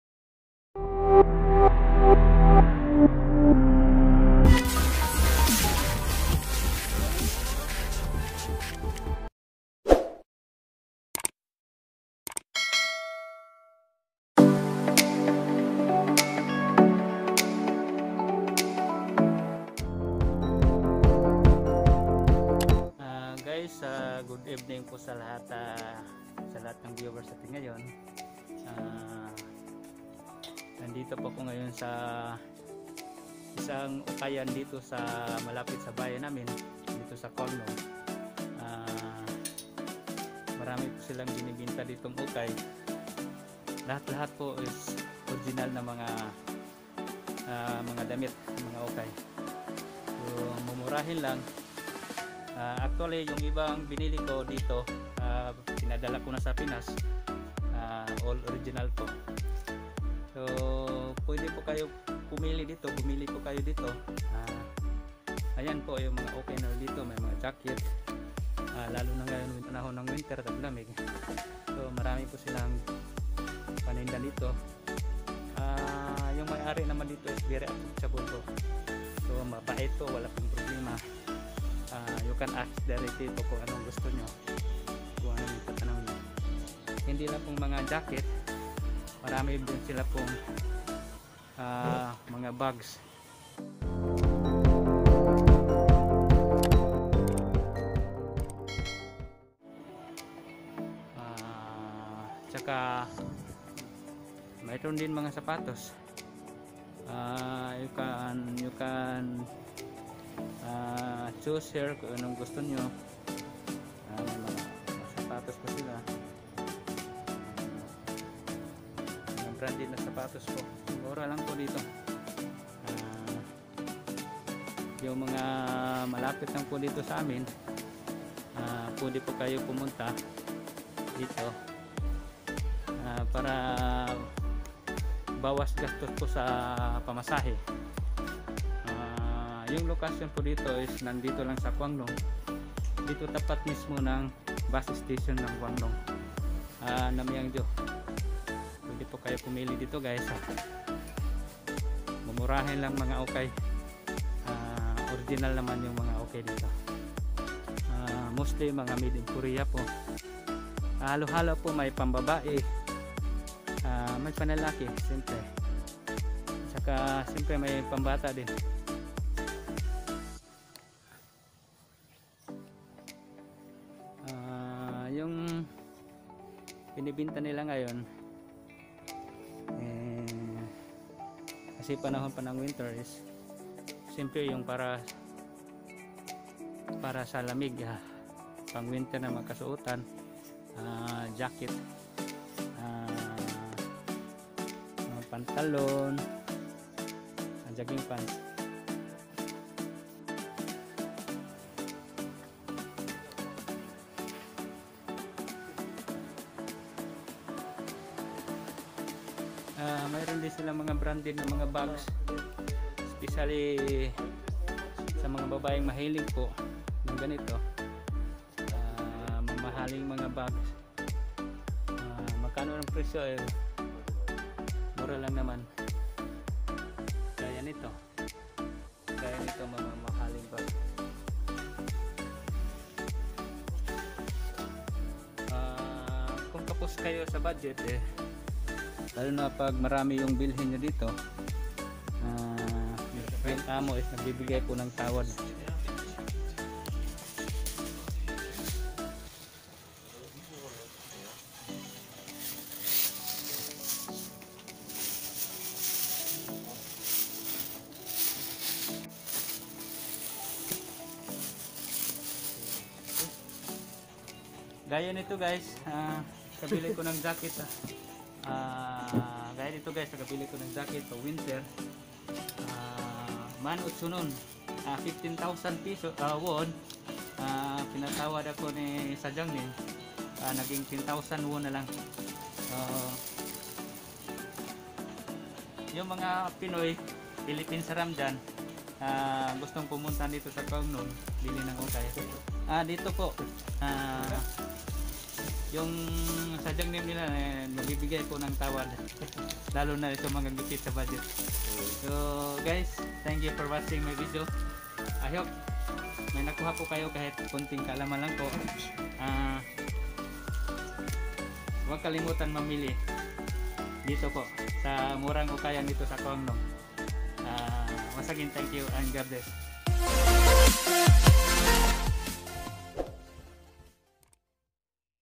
SOUTH KOREA:MURANG UKAYAN buy many for less spend pa
murang ukayan dito sa kwangneung, namyangju south korea may tawad pa pag bulk buy.
branded na ukay.
super mabait pa yung may ari.